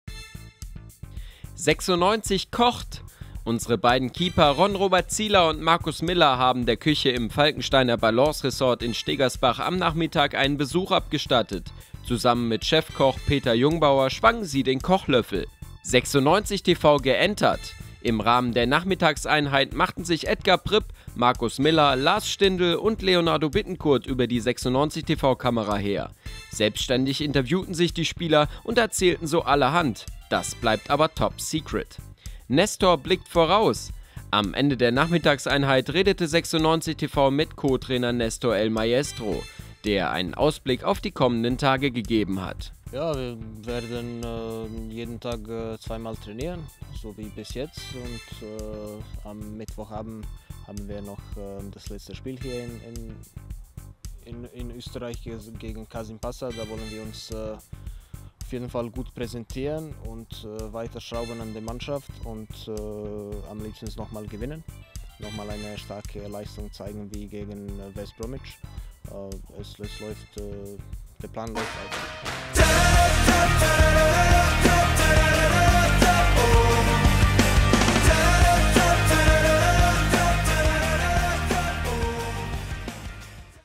TL Stegersbach | 96-News | Tag 5
Am fünften Tag im Trainingslager von Hannover 96 wurde gekocht, geentert und Co-Trainer Nestor El Maestro verriet, was in den kommenden Tagen so alles auf dem 96-Plan steht.